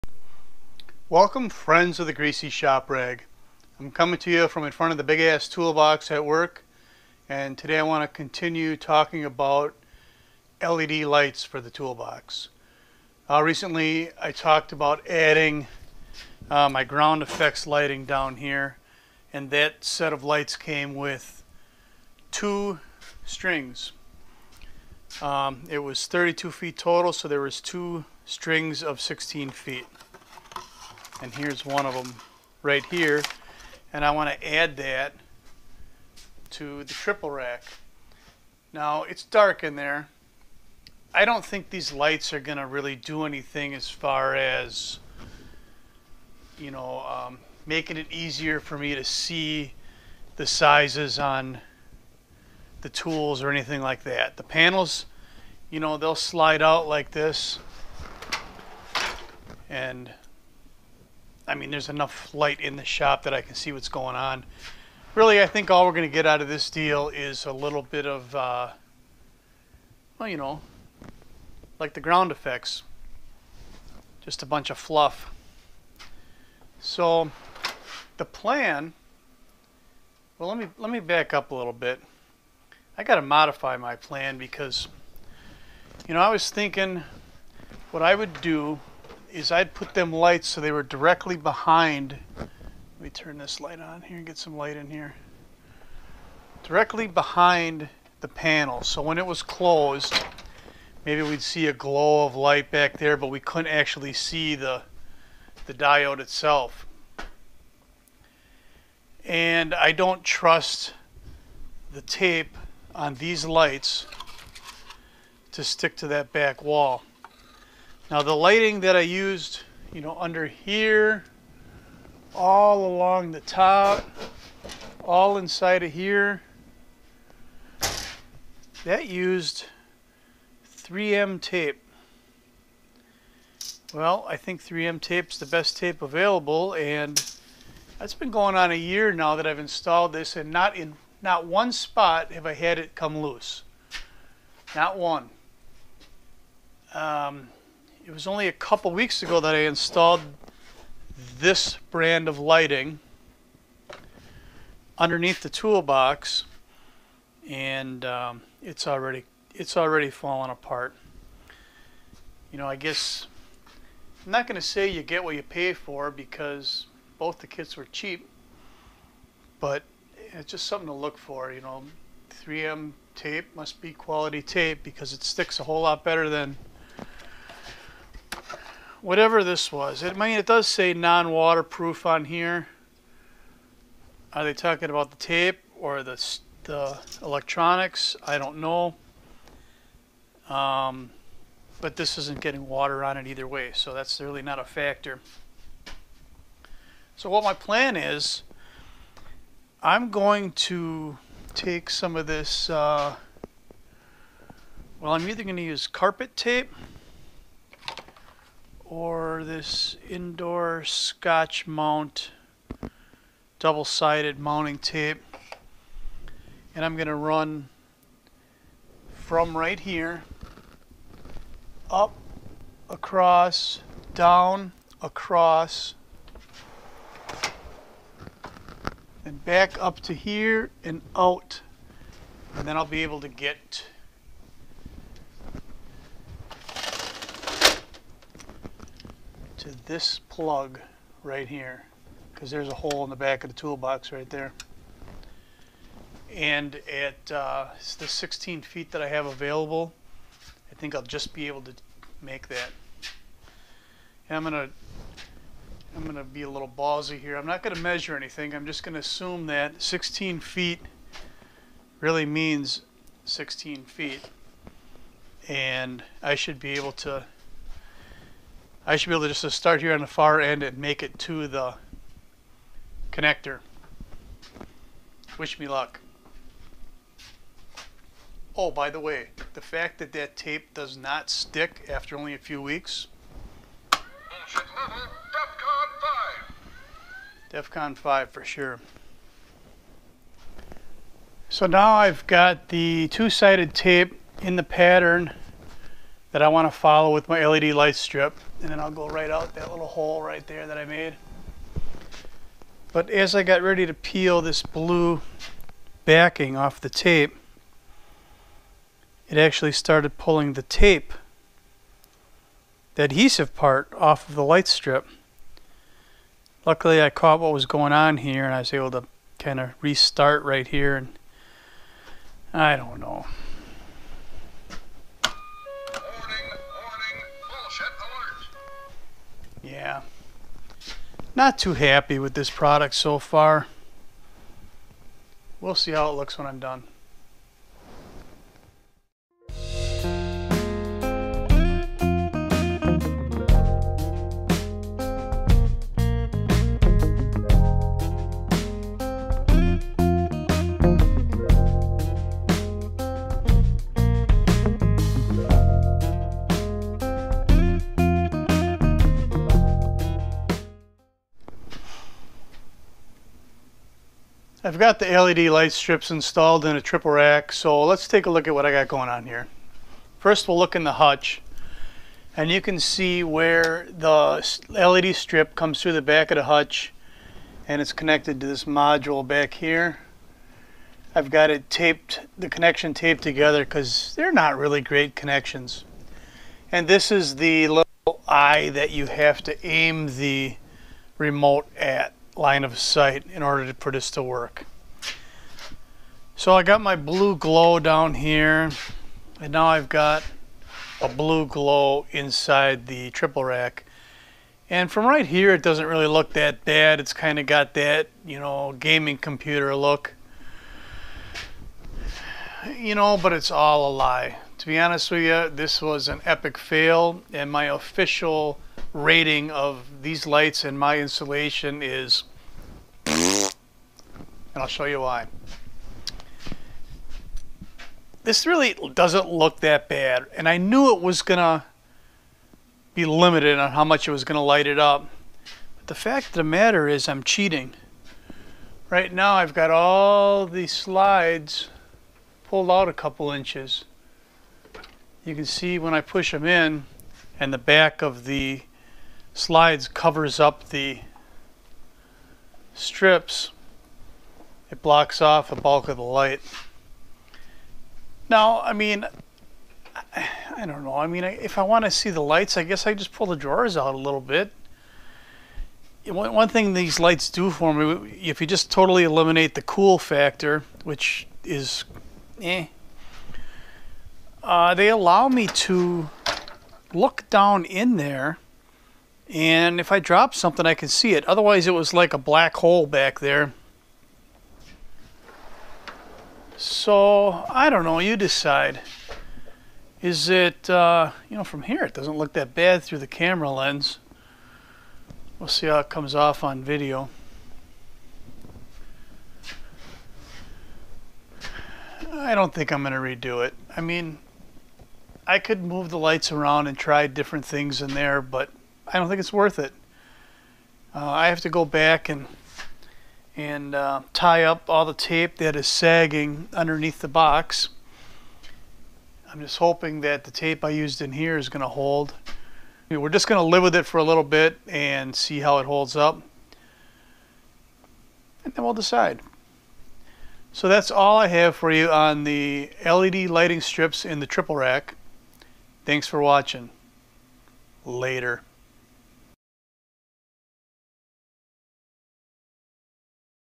Even More LED Lighting!
A continuation of the toolbox LED lighting adventure. This time I continue the light strip into the triple rack side locker.